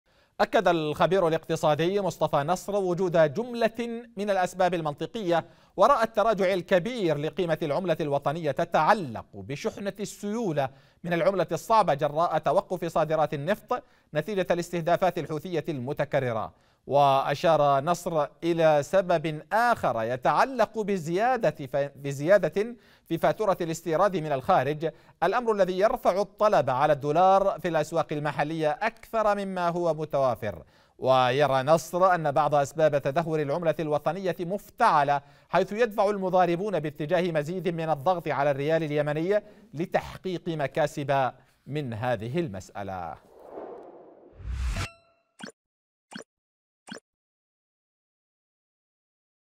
مصطفى نصر : المضاربون وتوقف صادرات النفط أسباب ساهمت في انهيار قيمة العملة الوطنية
قناة_سهيل# ‪